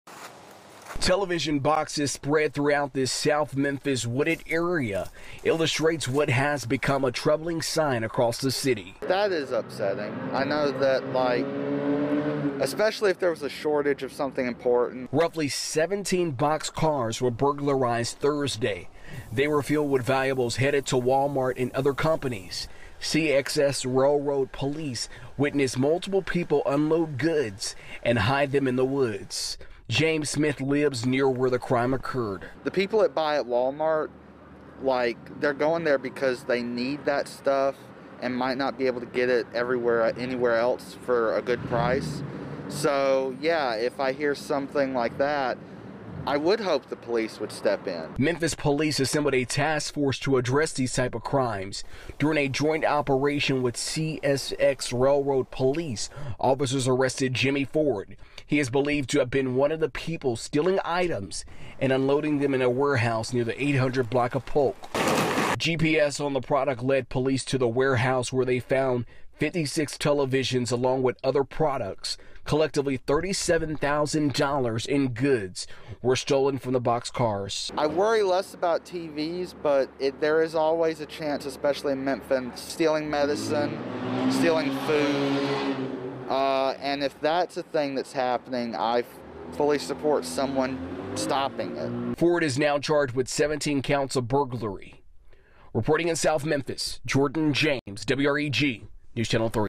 Memphis man busted in boxcar burglary sting
A Memphis man has been charged with 17 counts of burglary after an operation conducted by Memphis Police and CSX Railroad Police in response to a large number of train car thefts.